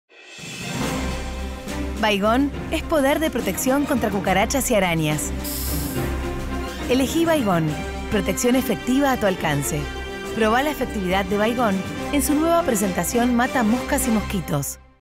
Publicidad Baygon Cucarachas y Arañas (2018) Argentina
Propaganda de Baygon
Mata Cucarachas y Arañas 2018

Marca: Baygon
Empresa: S.C Johnson & Son de Argentina

No sé el nombre de la Locutora

encontrado en VIMEO